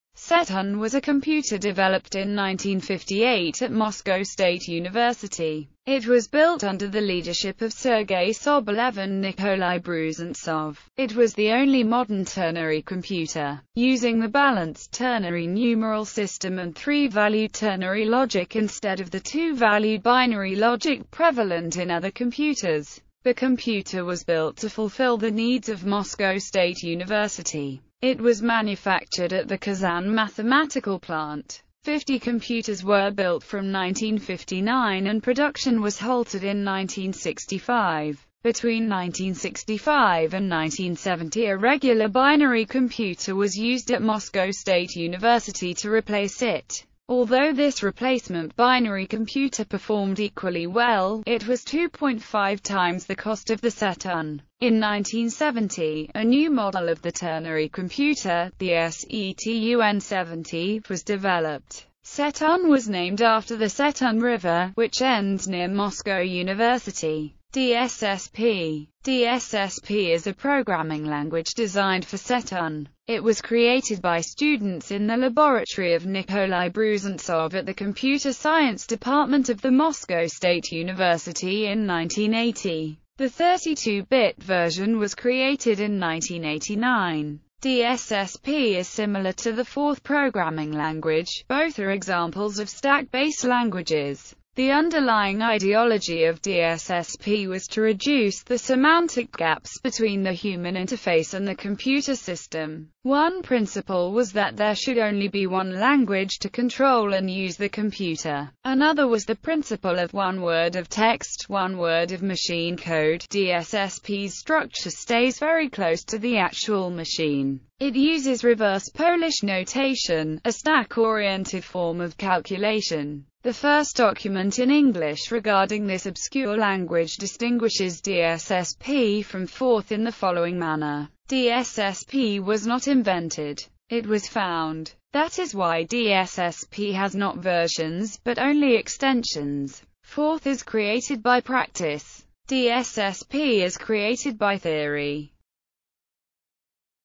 Setun
Setun (Russian: Сетунь) was a computer developed in 1958 at Moscow State University.It was built under the leadership of Sergei Sobolev and Nikolay Brusentsov.

Image-Copyright-Info
Image is in public domain
Author-Info:   Автор снимка с фотографии Андрей Куликов
Image-Copyright-Info